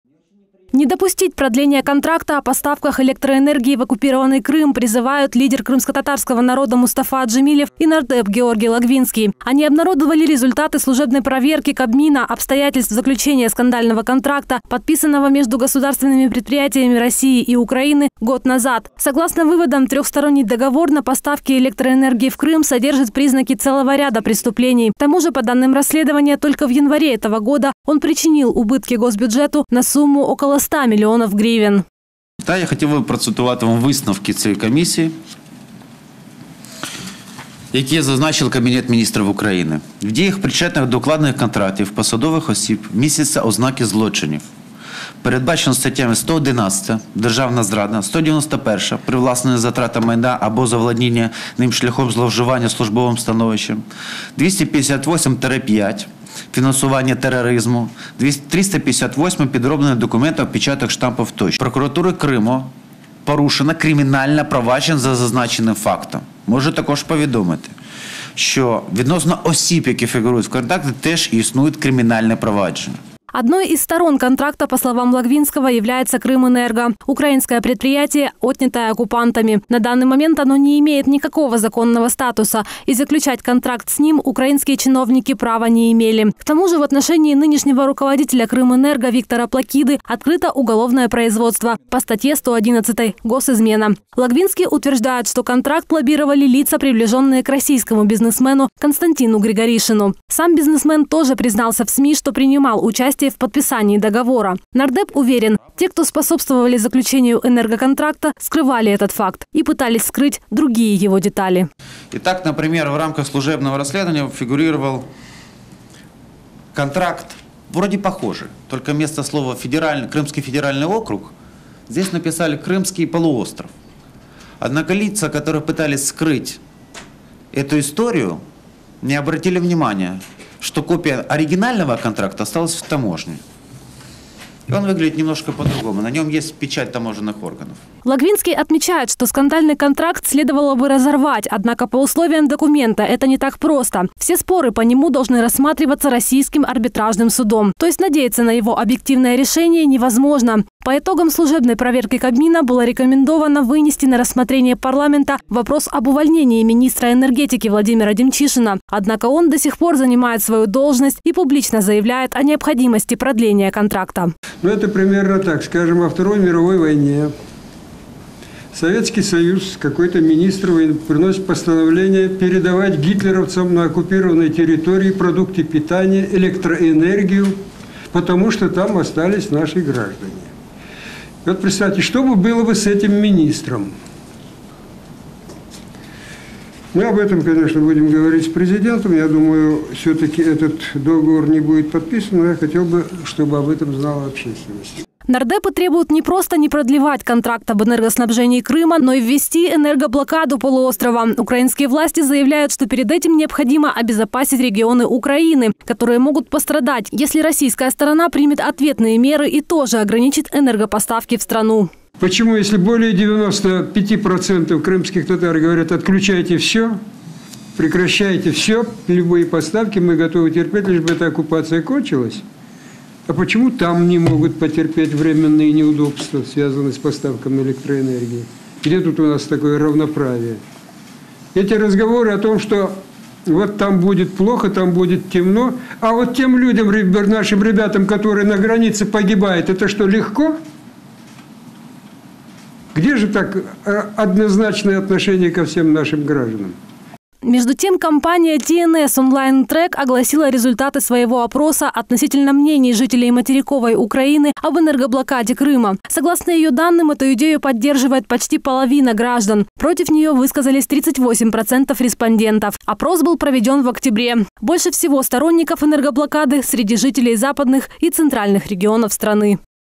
Народные депутаты требуют прекращения поставок электроэнергии в Крым
Согласно выводам служебной проверки Кабмина,  договор на поставки электроэнергии в Крым содержит признаки целого ряда преступлений. Подробности - в сюжете службы новостей "Волна" на ЧТРК.

Мы рядом!